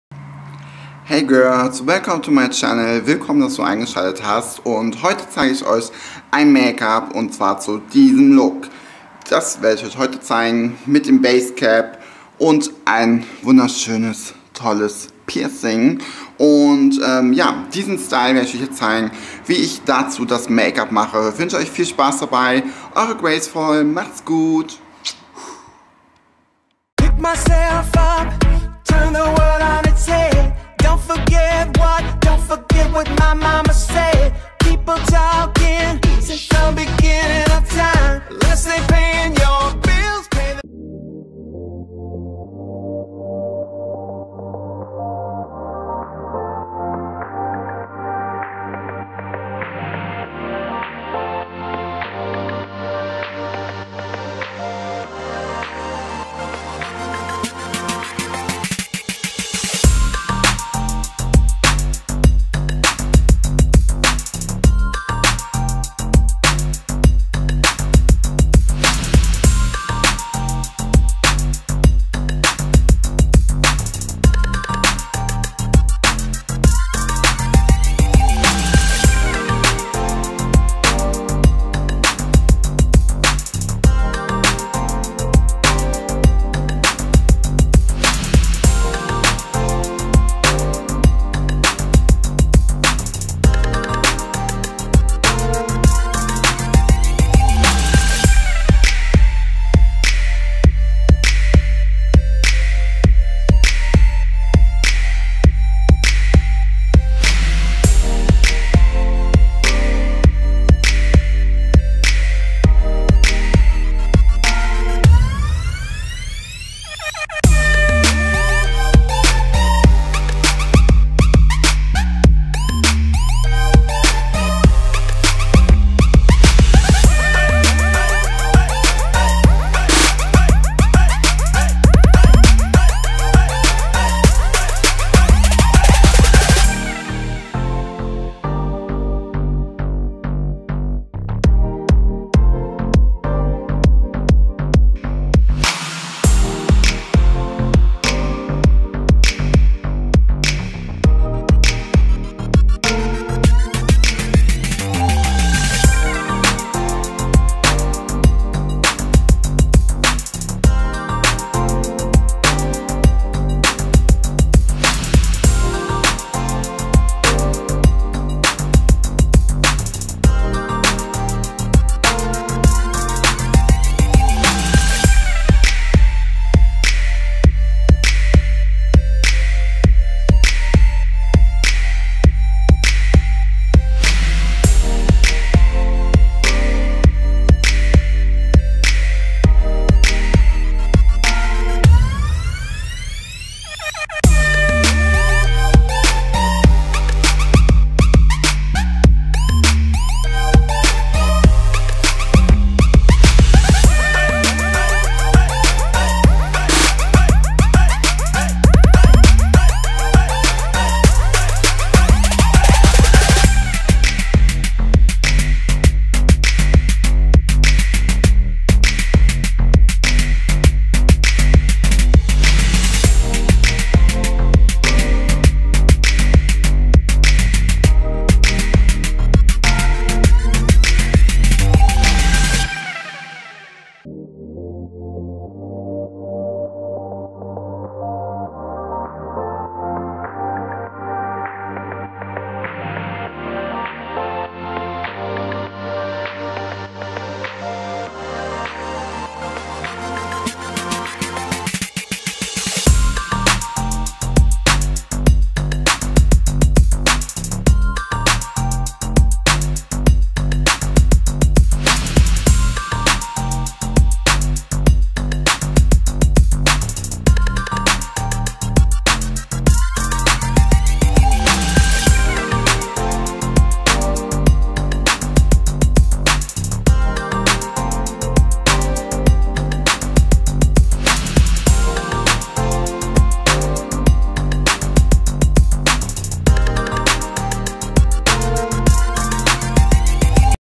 NEW make-up LOOK
Hey Girls,

In dem Video zeige ich euch meinen neuen Make-up look zum Thema basecap ;)

Freue mich sehr über eure Kommentare sowie über eure Likes...

in Liebe eure
Graceful